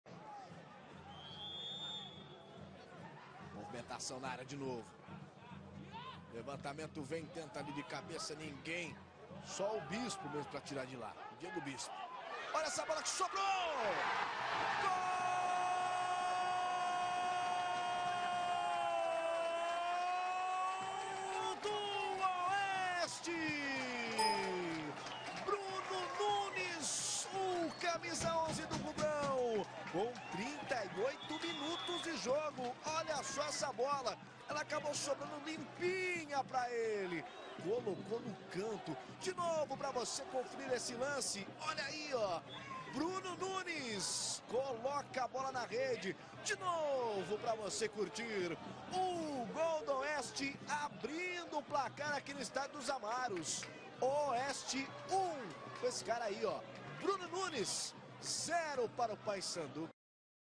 Oeste 1x0 Paysandu - Série B 2013
Confira o gol Oeste 1 x 0 Paysandu

OESTE 1 X 0 PAYSANDU
Local: Estádio dos Amaros, em Itápolis (SP)
Data: 13 de agosto de 2013, terça-feira
Horário: 19h30 (de Brasília)
Árbitro: Renato Cardoso da Conceição (MG)
Assistentes: Luiz H. de Souza Santos Renesto (PR) e Esdras Mariano de Lima Albuquerque (AL)
Cartões amarelos: Lelê e Bruno Sabino (Oeste); Raul, Diego Bispo, Esdras, Zé Antônio e Marcelo Nicácio (Paysandu)
Gols:
OESTE: Bruno Nunes, aos 36 minutos do primeiro tempo

OESTE: Fernando Leal; Eric (Neto), Dezinho, Ligger e Fernandes; Wanderson, João Denoni, Everton Dias e Bruno Sabino; Bruno Nunes e Emerson (Pablo).
Técnico: Edison Só

PAYSANDU: Marcelo; Yago Pikachu, Diego Bispo, Fábio Sanches (Raul) e Pablo; Esdras (Alex Gaibu), Zé Antônio (Iarley), Diego Barboza e Ricardo Capanema; Marcelo Nicácio e Heliton.
Técnico: Arturzinho